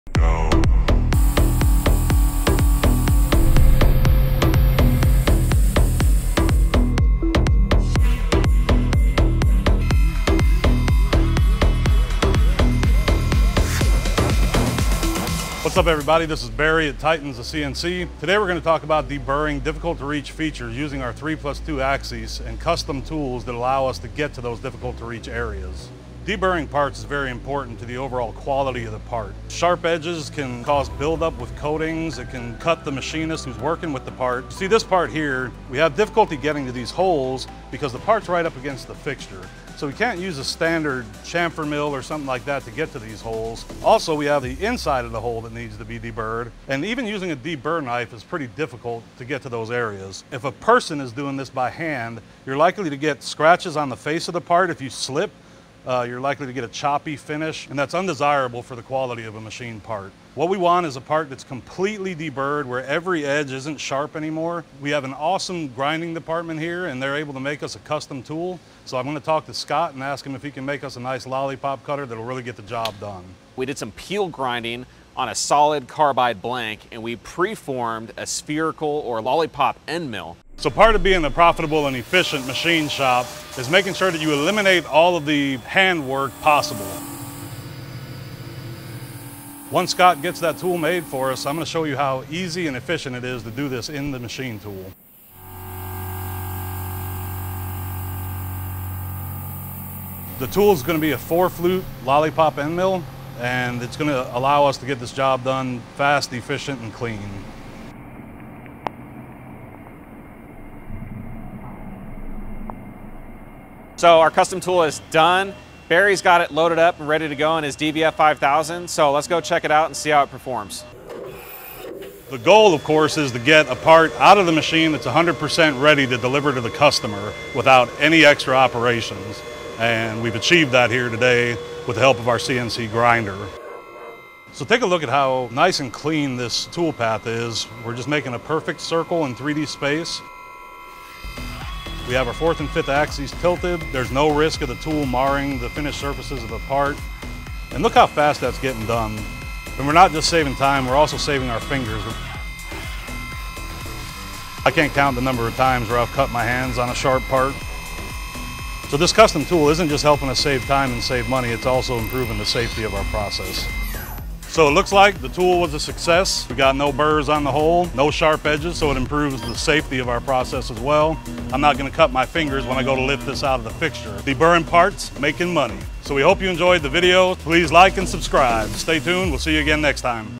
Using Custom Tool Made In-House to Machine Parts on Trick 5-Axis Fixture
Scott Makes a custom Lollipop End Mill on the new Walter CNC Grinding Machine and Barry uses it to do some trick deburring on production parts.

Help support FREE industrial education by purchasing the products shown in our videos Easy purchasing, at a fair price.

MB017E7JAFBH4VC